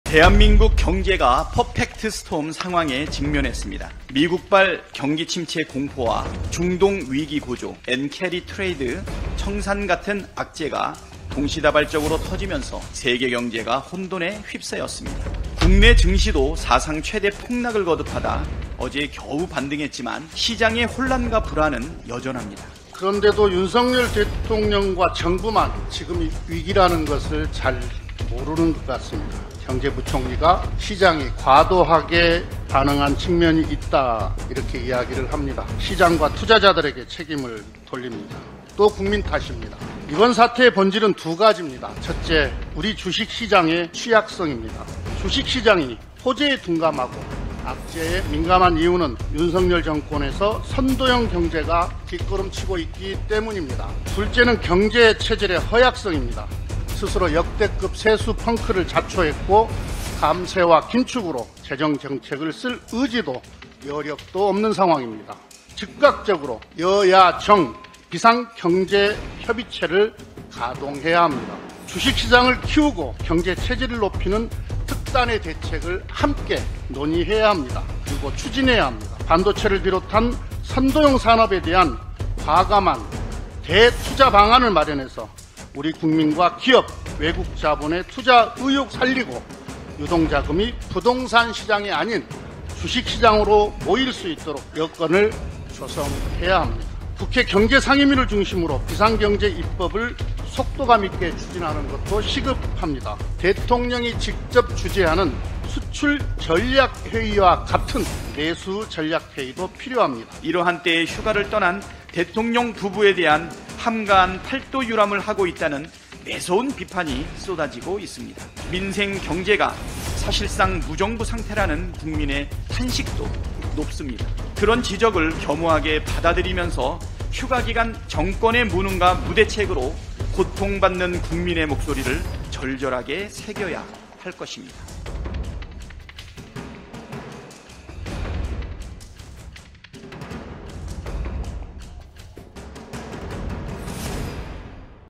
경제는 '비상'인데 대통령은 한가한 '팔도유람'
대한민국 경제가 ‘퍼펙트 스톰’ 상황에 직면했습니다.  국내 증시도 사상 최대 폭락을 거듭하다 겨우 반등했지만, 시장의 혼란과 불안은 여전합니다. 그런데도 윤석열 대통령과 정부만 지금 위기라는 것을 잘 모르는 것 같습니다.